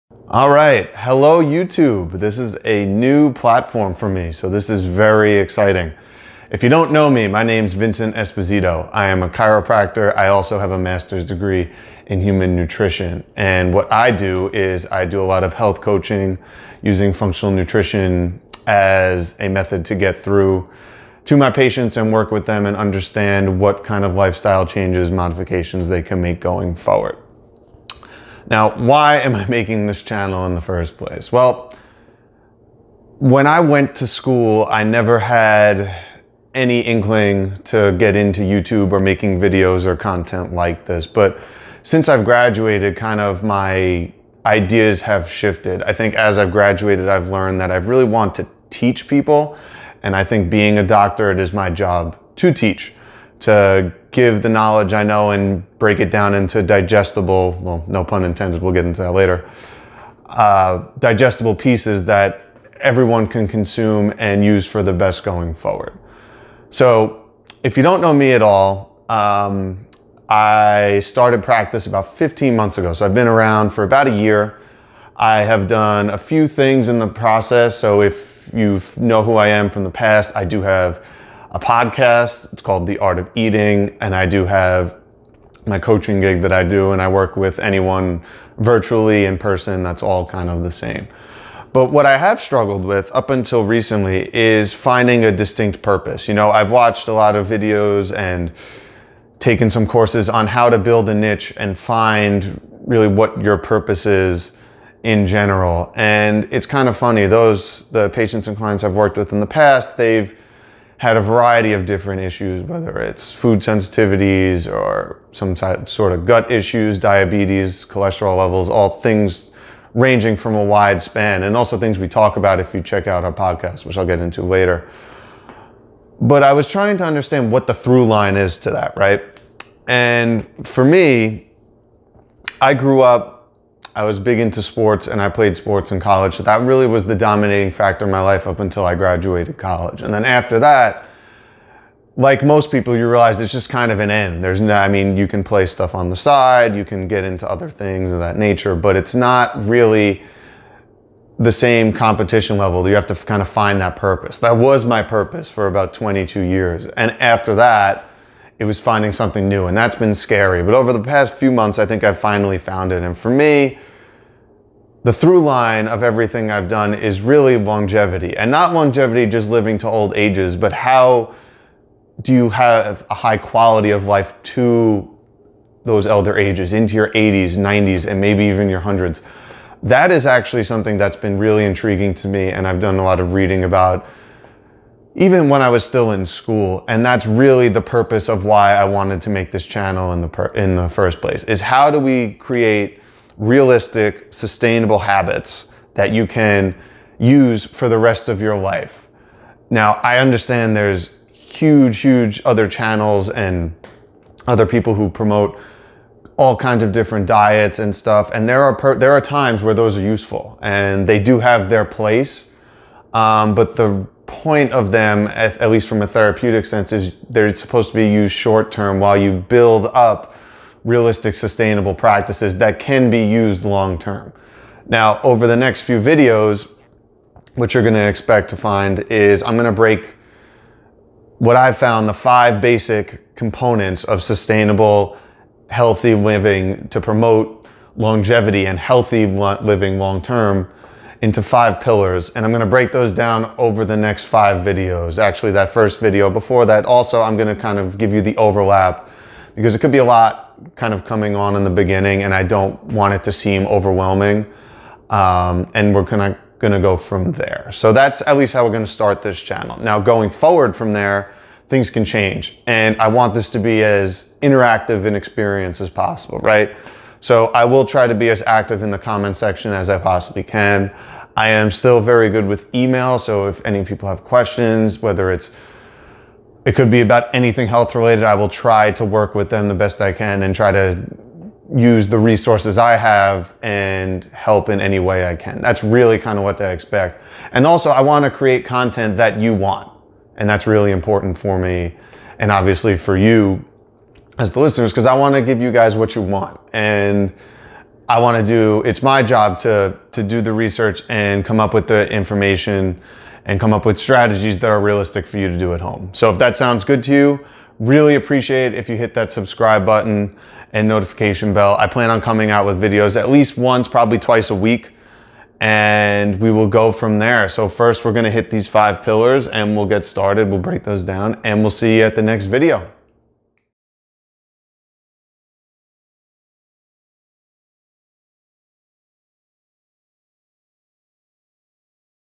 Hello YouTube! Welcome to the Channel!
Hello YouTube!  Welcome to the channel!  My name is Vincent Esposito, MS, DC, and my goal is to restore your health and maximize your life to its fullest potential by promoting long-lasting, sustainable, healthy, lifestyle modifications that allow you to be at your best for years to come.

If you find this information interesting...

Listen to our podcast, The Art of Eating, and check out our courses at evokhealth.com.

Want to get in touch?  Reach out!